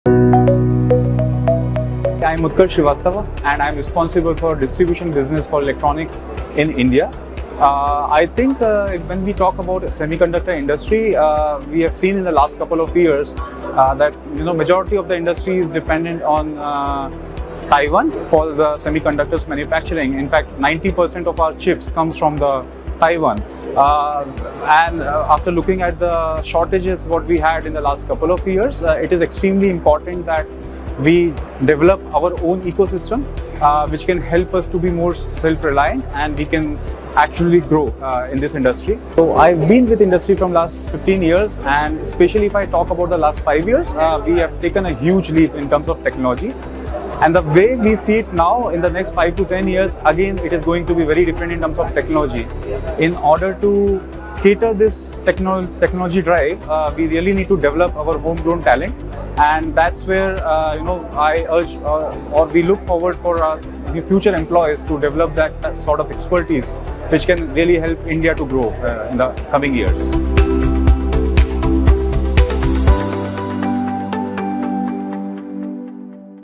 Utkarsh Srivastava
WATCH | India is witnessing a major transformation in the tech sector. Here's a message from an industry leader for India's techspirants!